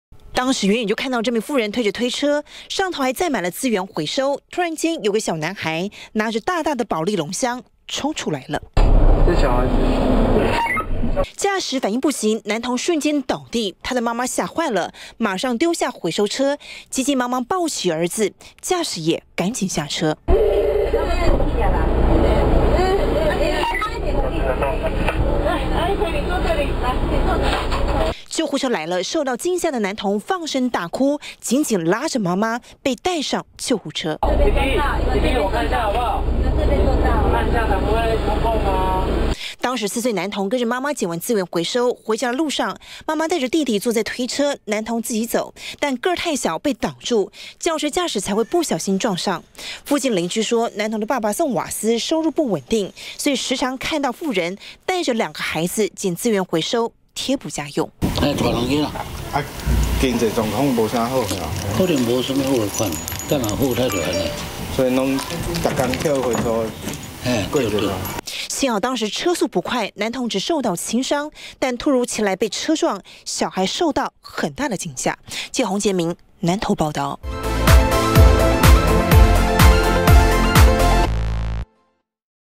20161218中天新聞 4歲童陪撿回收 個兒太小 轎車撞飛
南投一名4歲男童，因為爸爸送瓦斯，收入不穩，所以媽媽撿資源回收，貼補家用，就在17號下午4點多，男童跟著媽媽去撿完資源回收，回家途中，媽媽用推車推著回收物走在馬路邊，男童卻突然衝過馬路，被轎車撞上，幸好車速不是太快，男童送醫後，沒有大礙。